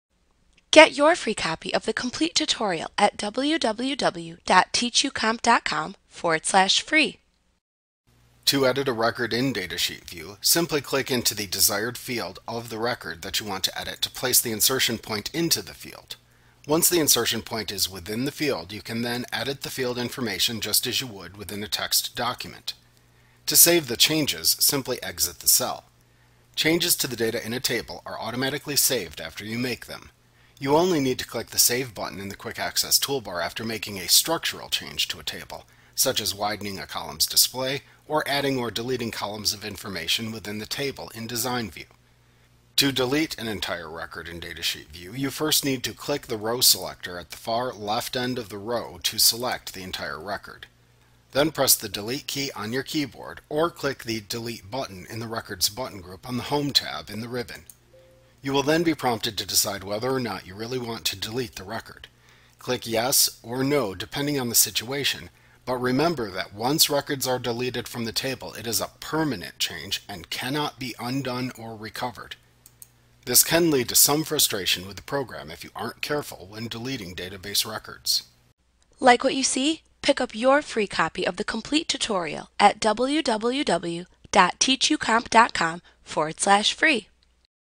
Access 2016 Tutorial Editing and Deleting Records in Datasheet View Microsoft Training
Learn about editing and deleting records in datasheet view in Microsoft Access - the most comprehensive Access tutorial available. Visit us today!